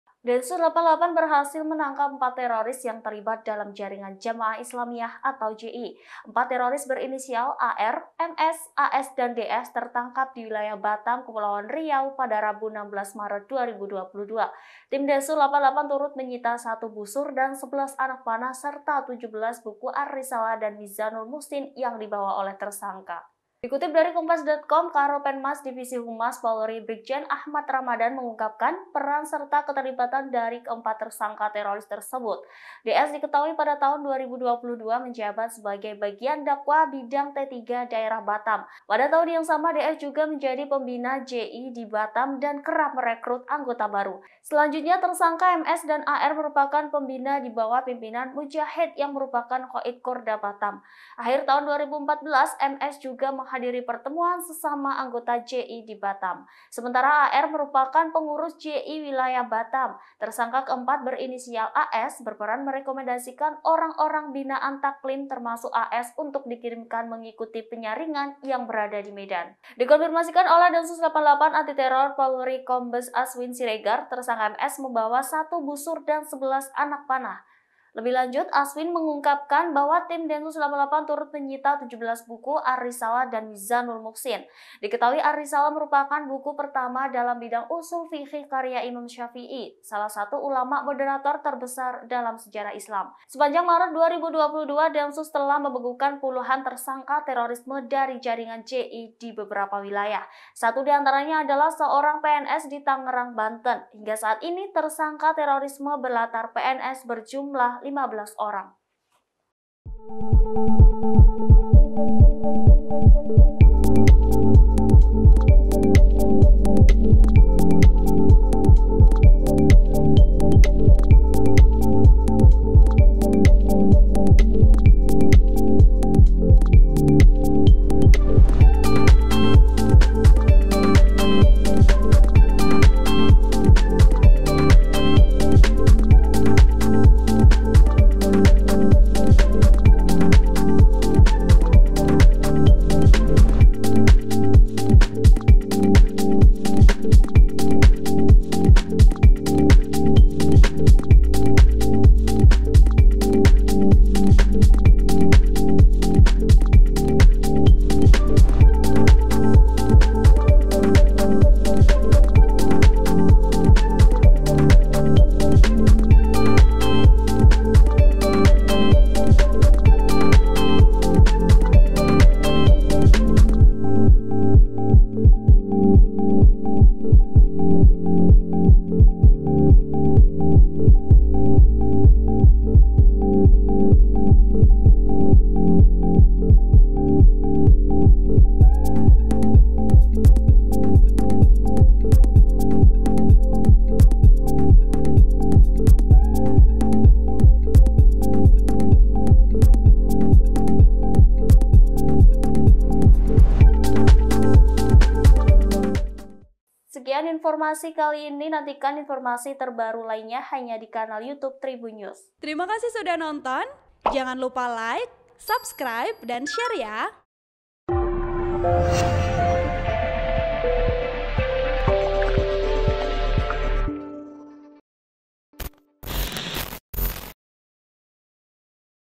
Densus 88 Tangkap Empat Tersangka Teroris di Batam, Sita Busur dan 11 Anak Panah Serta 17 Buku
TRIBUN-VIDEO.COM - Densus 88 berhasil menangkap empat teroris yang terlibat dalam jaringan Jamaah Islamiah (JI).

Empat teroris berinisial  AR, MS, AS, dan DS tertangkap di wilayah Batam, Kepulauan Riau (Kepri) pada Rabu (16/3/2022).